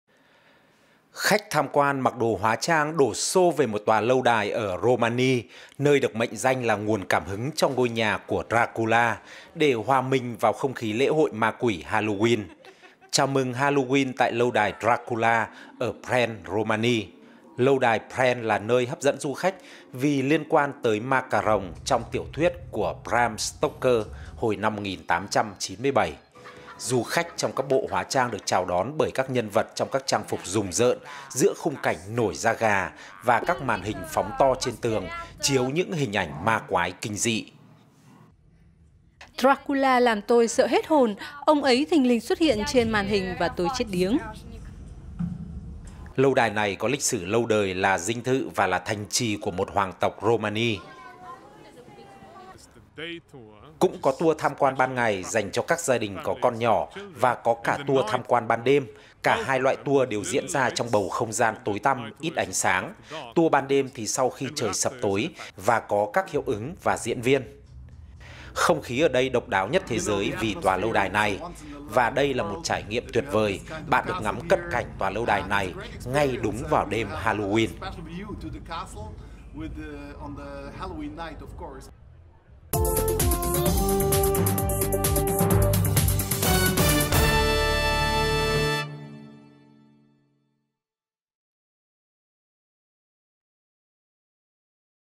Chơi Halloween trong lâu đài ma cà rồng Dracula | VOA Tiếng Việt
Khách tham quan mặc đồ hóa trang đổ xô về một tòa lâu đài ở Romani, nơi được mệnh danh là nguồn cảm hứng cho ngôi nhà của Dracula, để hòa mình vào không khí lễ hội ma quỷ Halloween.
Chào mừng Halloween tại ‘lâu đài Dracula’ ở Bran, Romani.

Lâu đài Bran là nơi hấp dẫn du khách vì liên quan tới ma cà rồng trong tiểu thuyết của Bram Stoker hồi năm 1897.

Du khách trong các bộ đồ hóa trang được chào đón bởi các nhân vật trong các trang phục rùng rợn giữa khung cảnh nổi da gà và các màn hình phóng to trên tường chiếu những hình ảnh ma quái kinh dị.

Em Sofia, du khách nói:

“Dracula làm tôi sợ hết hồn. Ông ấy thình lình xuất hiện trên màn hình và tôi chết điếng.”

Lâu đài này có lịch sử lâu đời là dinh thự và là thành trì của một hoàng tộc Romani.

Ông Alexandru Priscu, quản lý tiếp thị Lâu đài Bran cho biết:

“Cũng có tour tham quan ban ngày, dành cho các gia đình có con nhỏ, và có cả tour tham quan ban đêm. Cả hai loại tour đều diễn ra trong bầu không gian tối tăm, ít ánh sáng, tour ban đêm thì sau khi trời sập tối và có các hiệu ứng và diễn viên.”

Ông Uwe, du khách từ Đức chia sẻ:

“Không khí ở đây độc đáo nhất thế giới vì tòa lâu đài này, và đây là một trải nghiệm tuyệt vời, bạn được ngắm cận cảnh tòa lâu đài này ngay đúng vào đêm Halloween.”